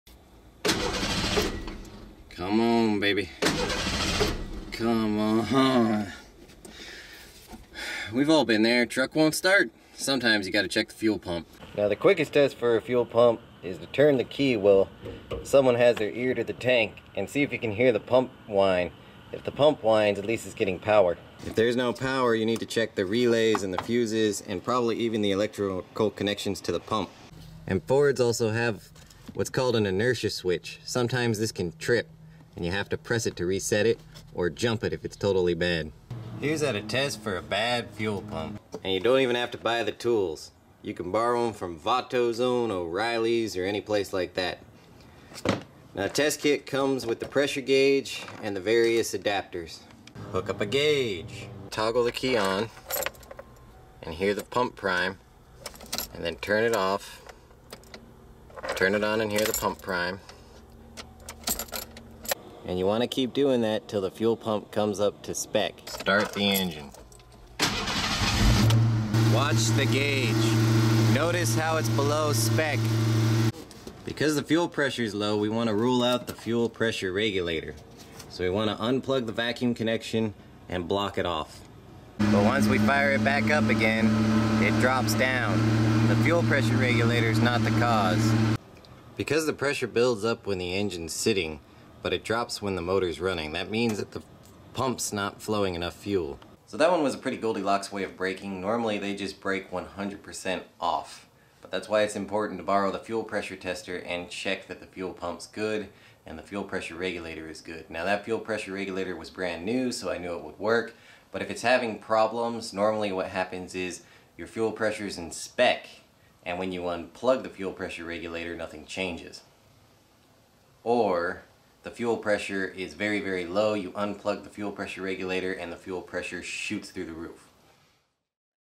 Pressure Testing an In-tank Fuel Pump (Ranger V6 all 1988-94)
I show how to troubleshoot and diagnose an in-tank fuel pump on the project 1991 ranger. This technique works for all vehicles new and old. You need to pressure test the pump so you know if it really is the issue and you aren't just wasting money!

I put the full write up on my blog.

Note: I know the fpr is good so I flubbed that part of the test. It should go up when unplugged. If not than its an issue. This pump just couldn't give it!

!!!Fuel pumps are not often the cause of your problem!!!

Basic steps:

Ensure the pump has Juice!

Hook Up gauge

Turn ignition on and off with 10 second pauses to let fuel pressure build (per book... you may just crank it). Clear the gauge to remove air (Step not in video)

Run engine and check pressure (pass/ fail)

Pass OR Fail - disconnect vacuum to Fuel Pressure regulator, pressure should jump to high spec. (Pass/Fail)

Readings:
Pass Pass - system OK

Pass Fail - Fuel Presser Regulator may be faulty

Fail Fail - Fuel Pump output low (Check lines for obstructions, replace fuel filter, etc. before just ordering the pump)

2nd test: NOT IN VIDEO
Raise pressure to spec, kill engine. Pressure should remain in SPEC for at least 1 minute (Fail/Pass)

Pass - system OK, no leaks

Fail - System leaks/ bad pump check valve/ faulty FPR

3rd test - NOT IN VIDEO
If gauge is moving a lot perform an "Air Test."

Build pressure in gauge, stick end of blow-off hose in an approved container with a bit of gas in it.

Purge the gauge slowly, Check for air bubbles (Pass/Fail)

Pass - no leaks

Fail - Leaks in fuel lines/ joints.